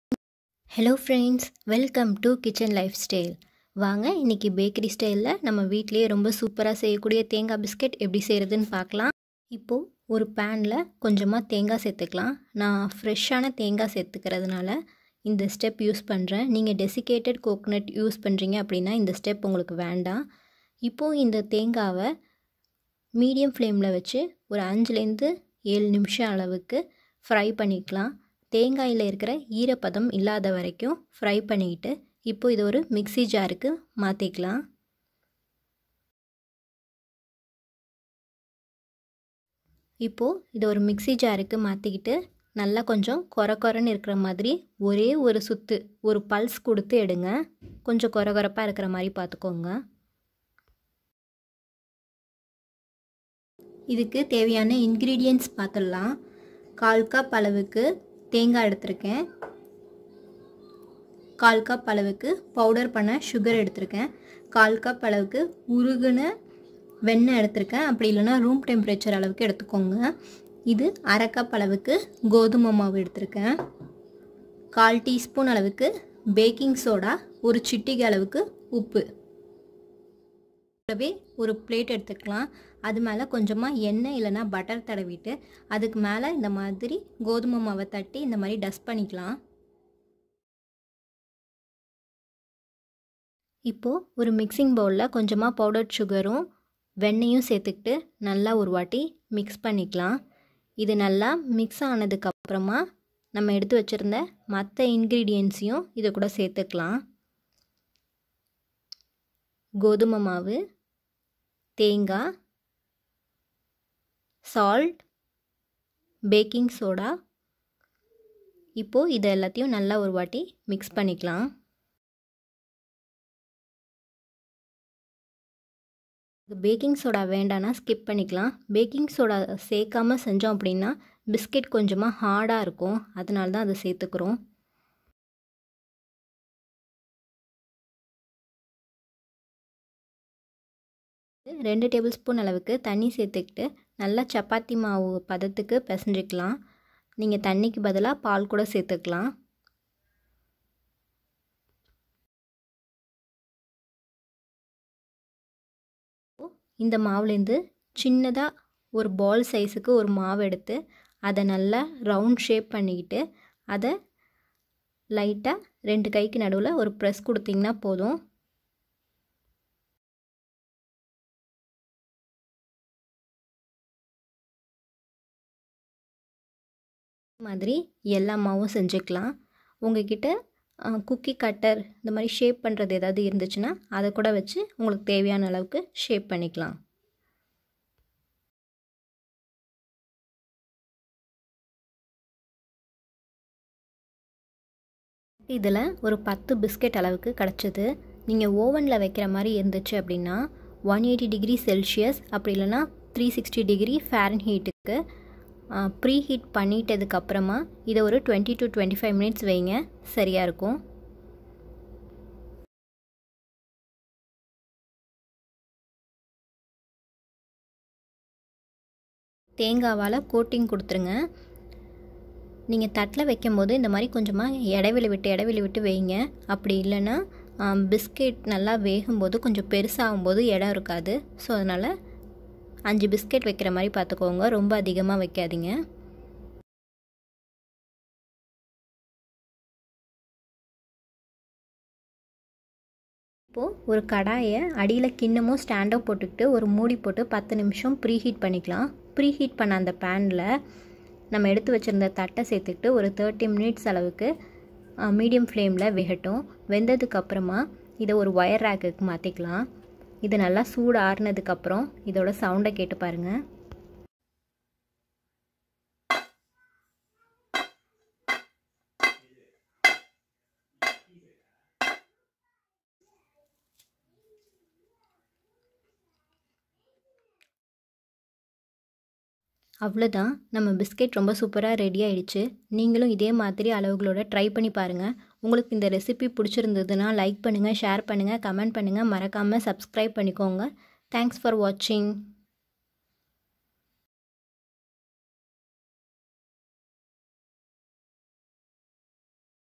கோதுமை மாவில் பேக்கரி சுவையில் தேங்காய் பிஸ்கட்|Coconut cookies recipe in tamil
Ingredients
1.coconut-1/4cup
2.butter-1/4cup
3.powderedsugar-1/4cup
4.wheat flour-1/2cup
5.milk/water-2tbsp
6.salt-pinch
7.baking soda-1/4tsp

Process
Mix all the ingredients
Add water or milk
Make chappathi dough like consistency
Make small ball
Press it with your palms
Shape the biscuit
Coat it with coconut

If you using oven
Preheat oven
For 180c/360f
20-25mins

If you are using gas stove
Preheat kadai with lid for 10 mins medium flame
Place the small bowl or stand
Place the biscuit cookies inside it
Medium flame
Bake it for 30 mins
Allow it to cool
Enjoy with coffee or tea..

If you really like my video